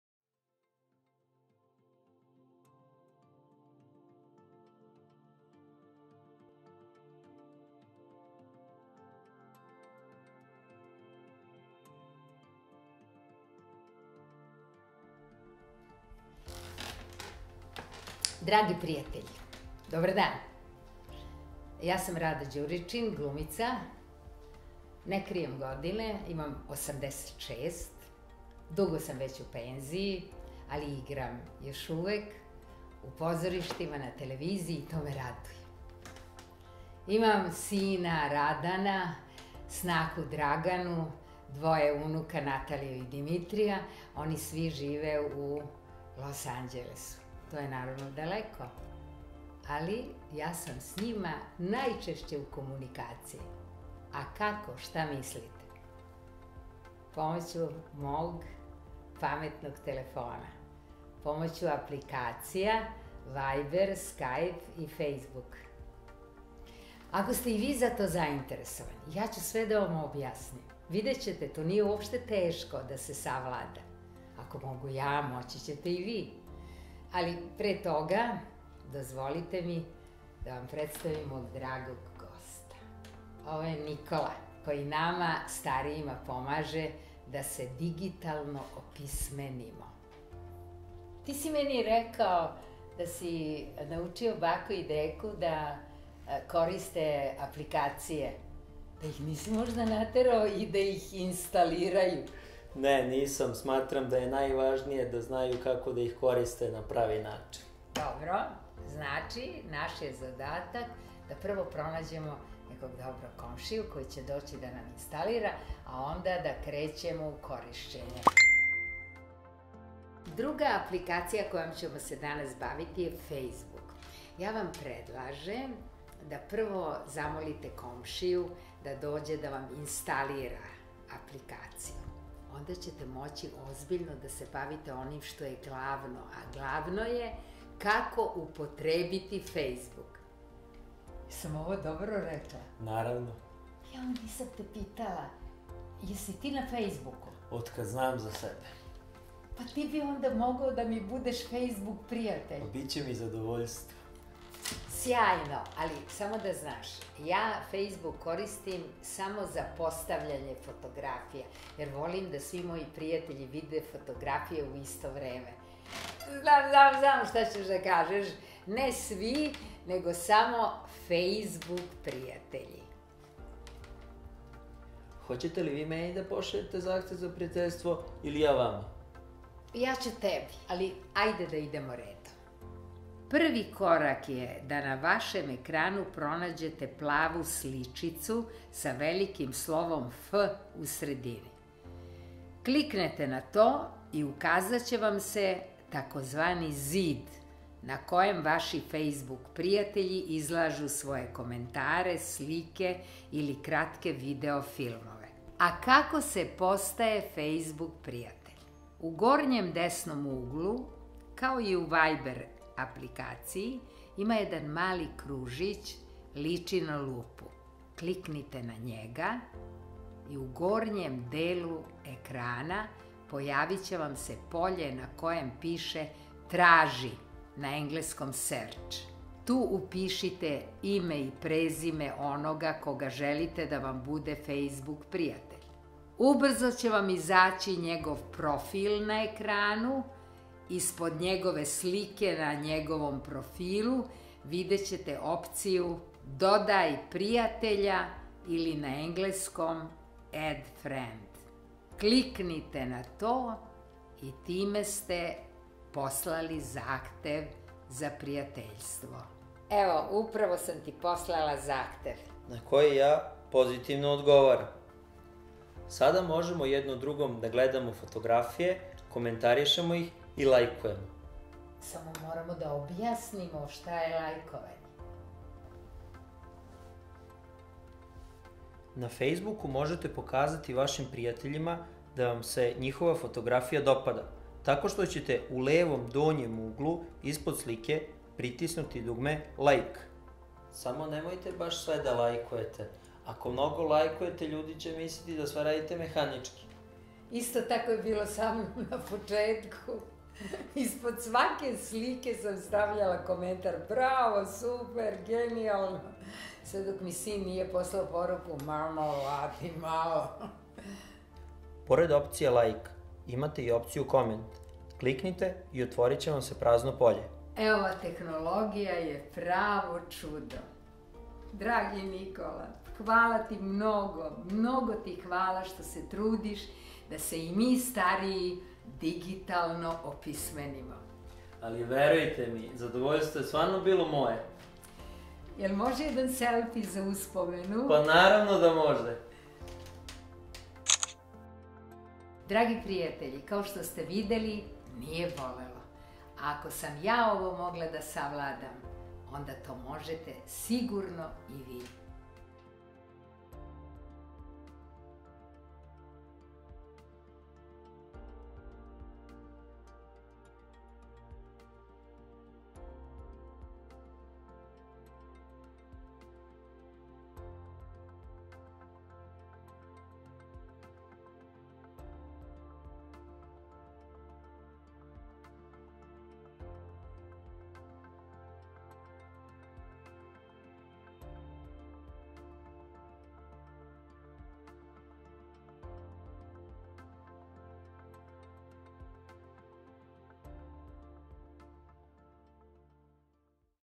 POVEŽI SE!: Kada sam ja uspela, zašto ne biste i vi? / Facebook
Video-tutorijal za korišćenje osnovnih aplikacija namenjenih starijim osobama
Za sve informacije i dodatna objašnjenja molimo vas da se javite na mejl . Članovi tima Mikser organizacije Vam stoje na raspolaganju za sva pitanja!

Uloge: Rada Đuričin, Nikola Đorđević
Režija: Đorđe Đurišić
Scenario: Ivan Lalić
Kamera i montaža: Petar Tkalec
Snimatelj zvuka: Damjan Plamenac
Prvi asistent reditelja: Ognjen Zaharijev
Producenti: Đorđe Đurišić, Jelena Pavlović, Petar Tkalec, Ognjen Zaharijev, Damjan Plamenac
Asistent produkcije: Goran Rajković
Dizajn: Branko Tmušić, Ksenija Pantelić

Film je podržan od strane Populacionog fonda UN (UNFPA)